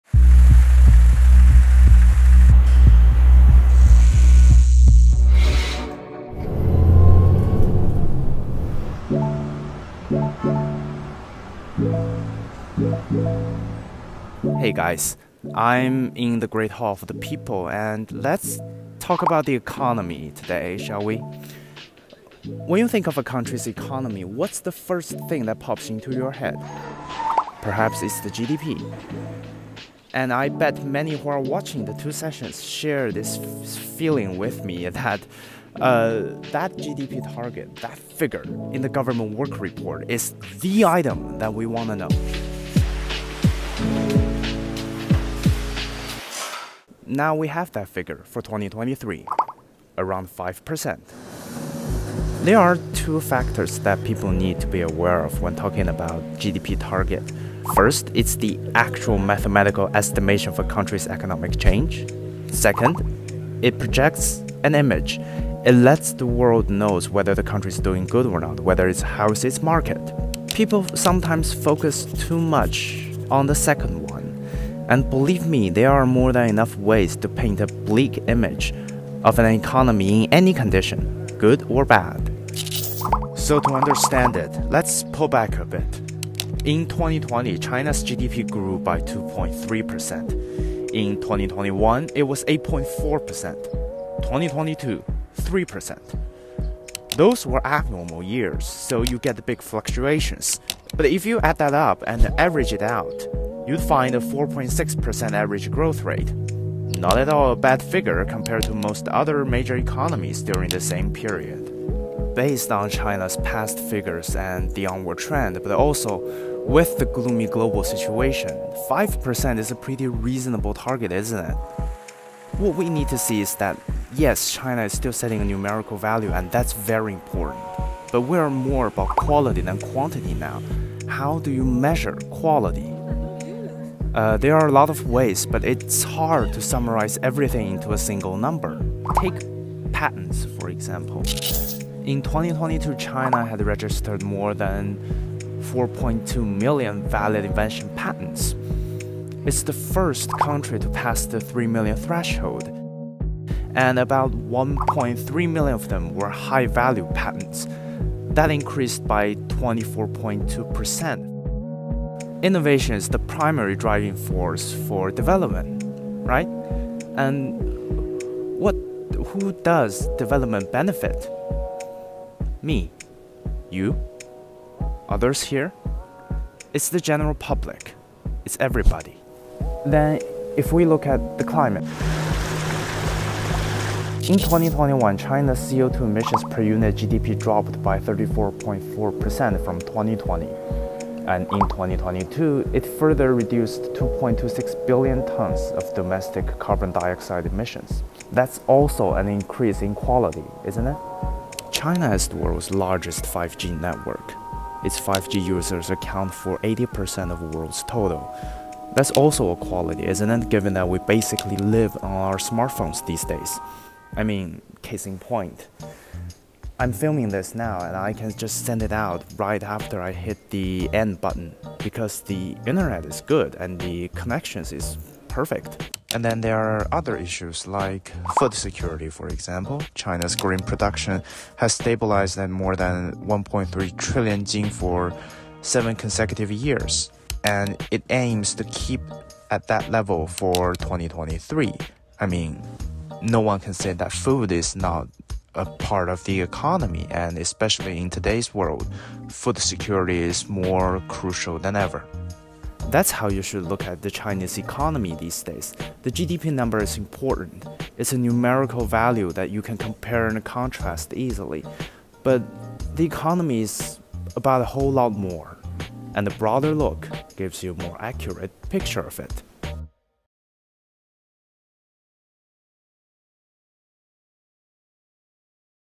Numbers matter, but more do real benefits for people
In this year's government work report, the GDP growth target for 2023 is set for around five percent. When foreign media look at the number, they keep painting a bleak image of the Chinese economy. So what does the five percent mean? From inside the Great Hall of the People, reporter Huang Jiyuan takes you out of the confinement of a single number, discusses the relationship between quantity and quality in economic development, and sees the Chinese economy from a broader perspective.